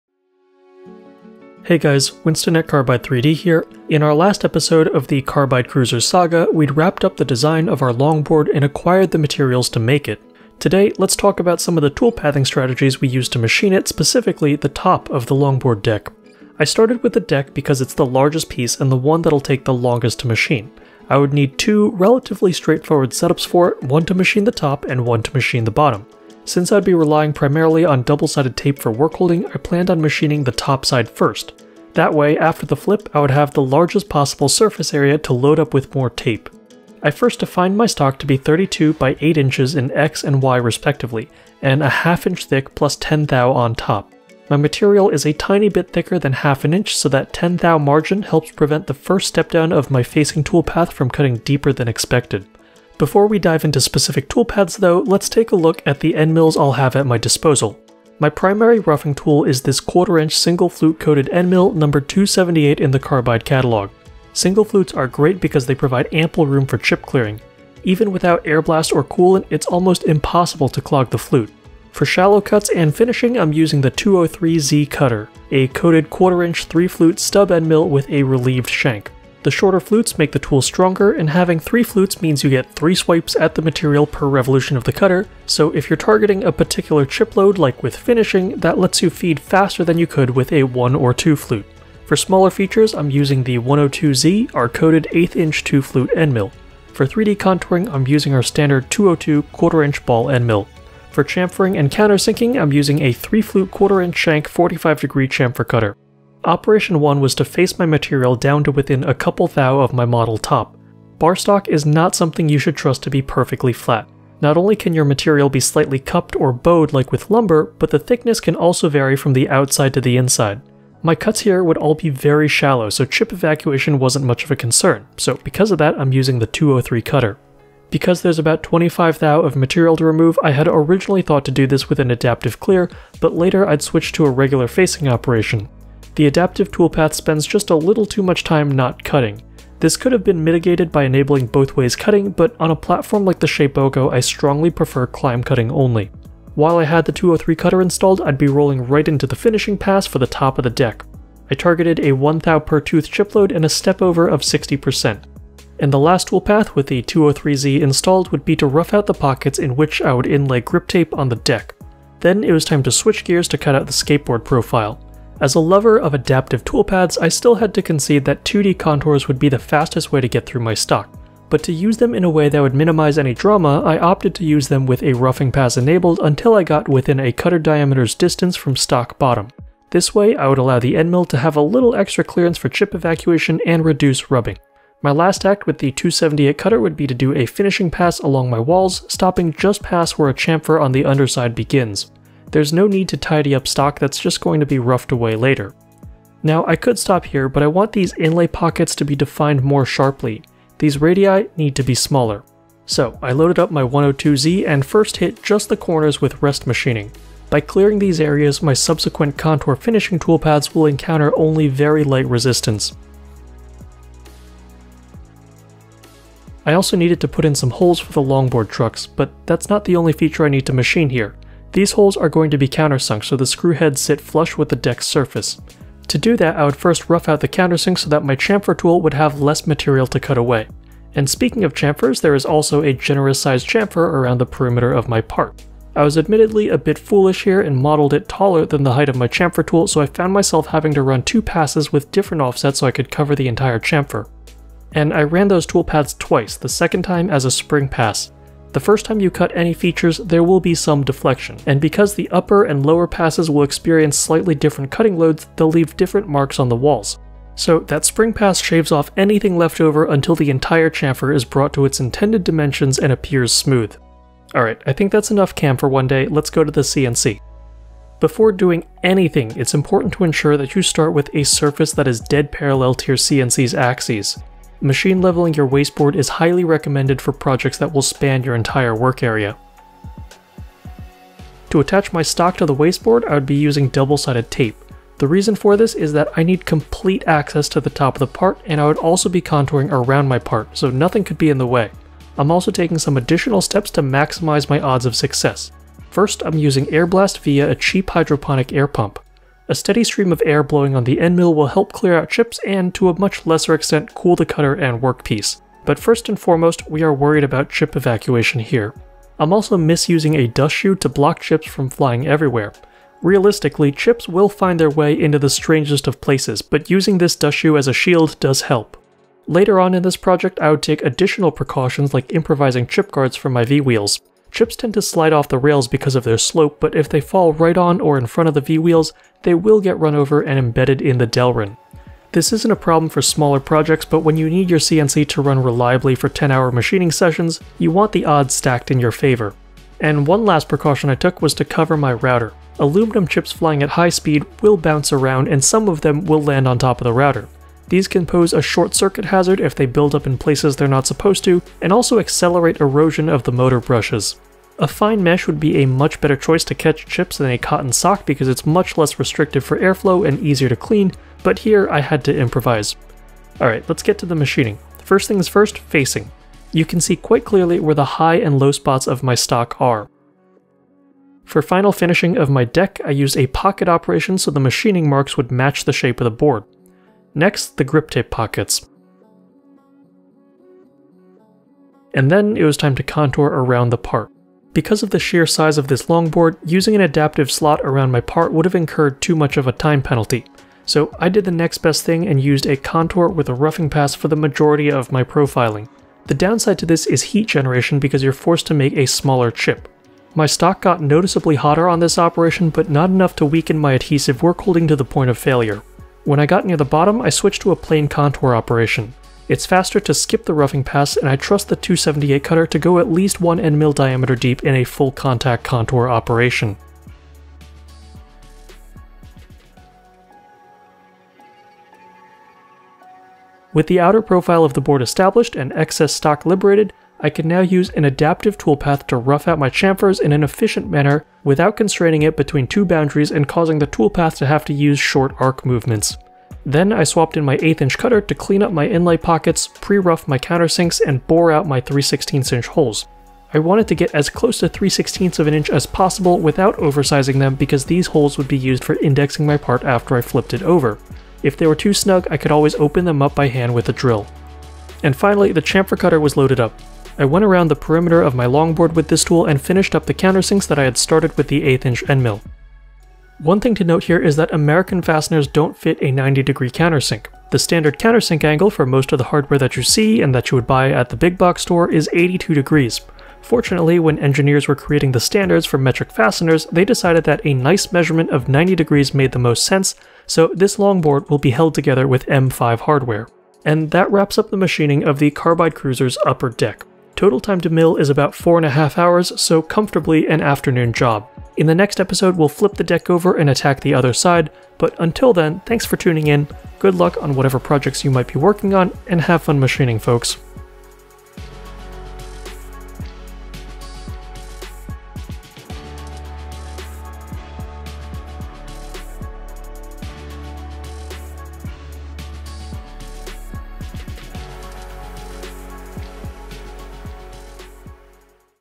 CAM and Machining of the Longboard Deck, Pt. 1 - Carbide Cruiser Ep. 2
A deep dive into the CAM and machining of the Carbide Cruiser Longboard's upper deck.

This project was machined on the Shapeoko 3 XL. To learn more about our line of CNC milling machines, check out